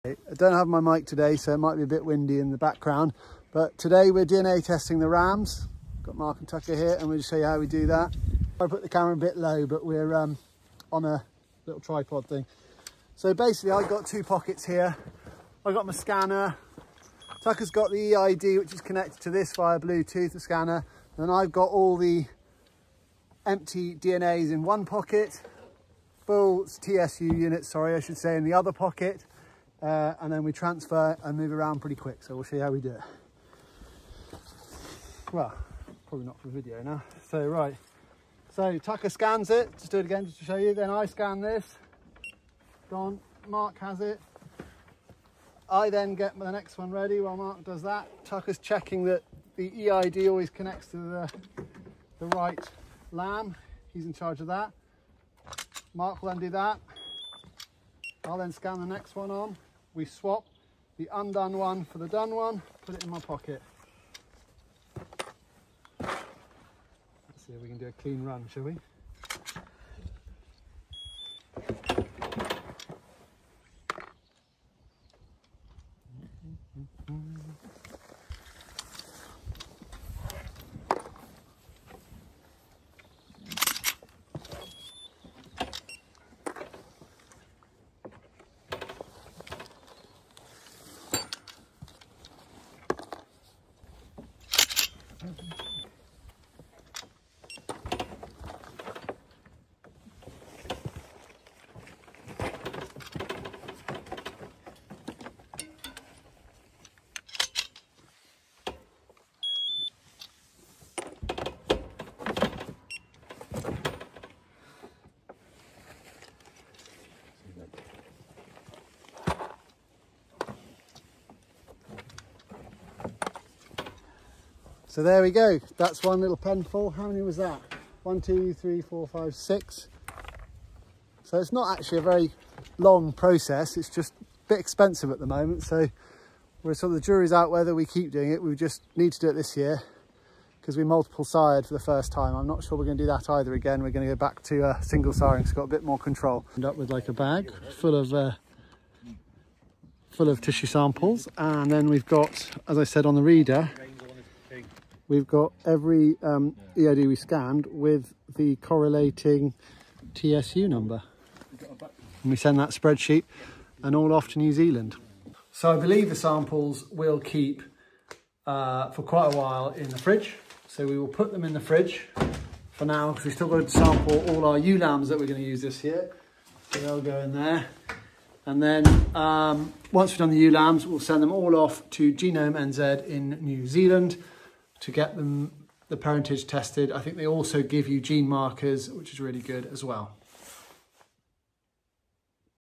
DNA SAMPLING LAMBS
A quick view of how we sample our wool shedding lambs for parentage using TSUs. we use a Tru test EID stick reader and connect that up via Bluetooth to a barcode/ qr code reader. We then send all samples to NZ to get parentage and gene markers.